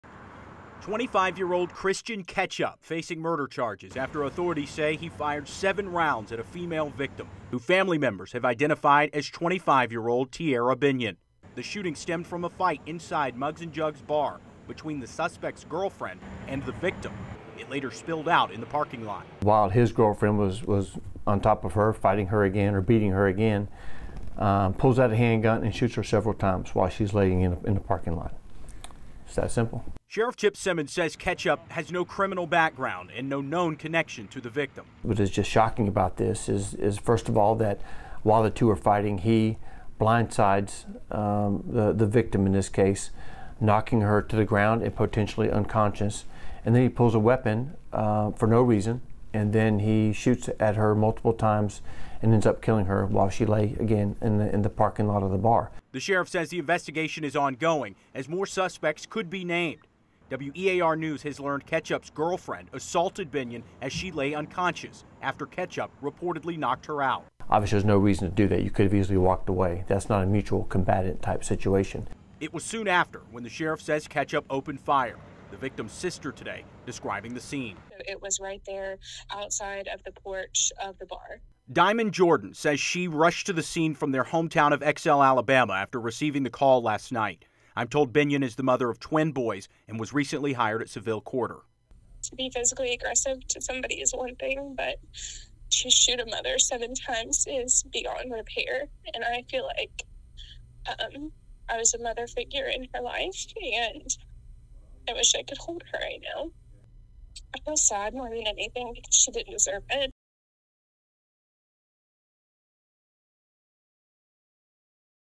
Mugs, Jugs, Ketchup and Murder... Pensacola Bar fight leads to homicide
August 23rd, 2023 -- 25-year-old Tierra Binion was shot and killed overnight Tuesday at Mugs & Jugs in Escambia County.

It happened around 1 a.m. Wednesday at the bar on Scenic Highway. Family identifies the victim as Tierra Binion -- a mother of twins.

Christian Ketchup, 25, of Pensacola, is charged for the murder. He is being held in Escambia County Jail on no bond.

The sheriff says an argument first began between Ketchup's girlfriend and Binion inside the bar. They then went outside to the parking lot and began fighting, which Sheriff Simmons described as "mutually combatant."

"During the fight, Christian Ketchup just jumps to [Binion] and punches her and knocks her aside," Sheriff Simmons said.

Binion was knocked out cold, according to the sheriff. Ketchup's girlfriend then started beating Binion on the ground, which is when Ketchup pulled out a handgun and shot Binion seven times.

Ketchup's girlfriend Rachel DeRise, 23, was charged with battery Thursday morning.